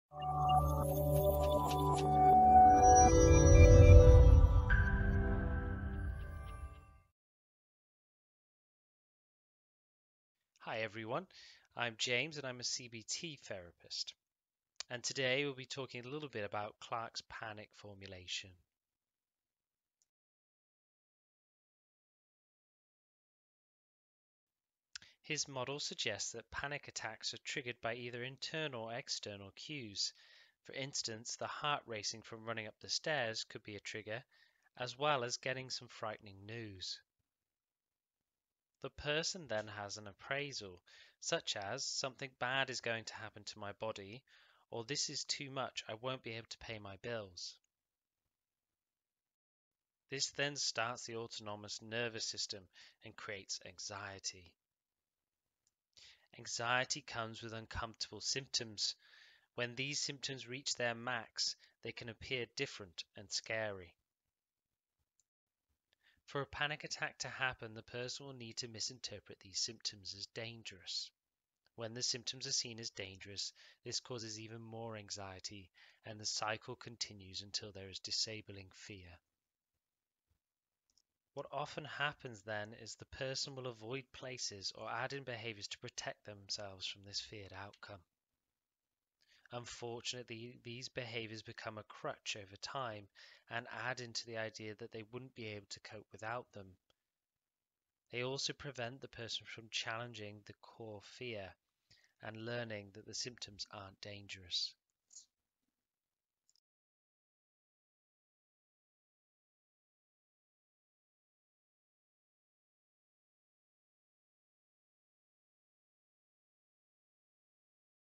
Panic Formulation - David Clark
This video explains Clark's model for understanding Panic Attacks.